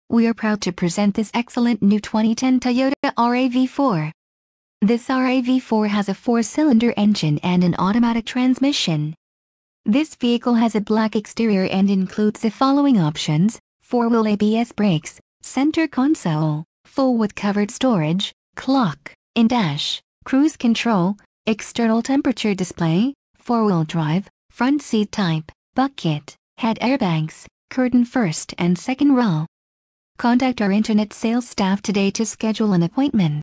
2010 Toyota RAV4 Wausau WI Toyota Of Wausau
Toyota Of Wausau
2900 N. 20th Avenue

2010 Toyota RAV4 Limited 4x4
Engine: 2.5L 4 cyls
Trans: Automatic 4-Speed
Color: Black